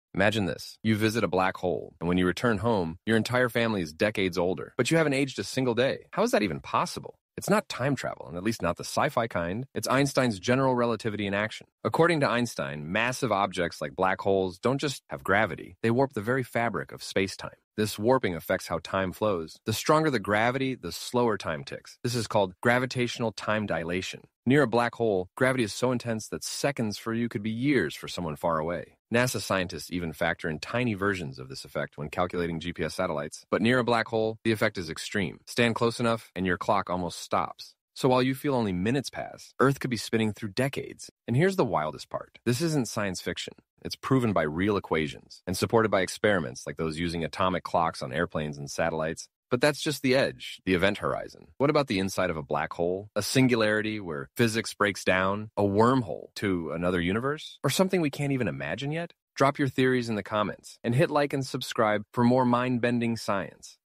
What Happens If You Fall Near a Black Hole?
What if you visited a black hole and returned home… only to find everyone decades older while you haven’t aged a day?
This isn’t science fiction — it’s real physics based on Einstein’s theory of General Relativity. Black holes don’t just have gravity — they bend spacetime, causing gravitational time dilation so intense that time nearly stops at the edge.
In this short, we break down the mind-bending truth about time near black holes and the mystery of what could lie inside: a singularity, a wormhole… or something we haven’t discovered yet.